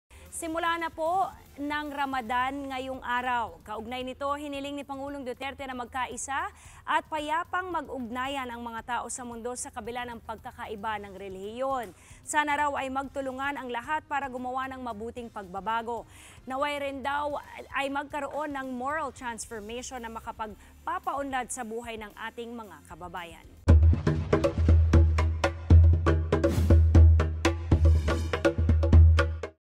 Ramadan 2019
Sumentro sa pagkakaisa at kapayapaan ang mensahe ni Pres. Rodrigo Duterte sa pagsisimula ng Ramadan.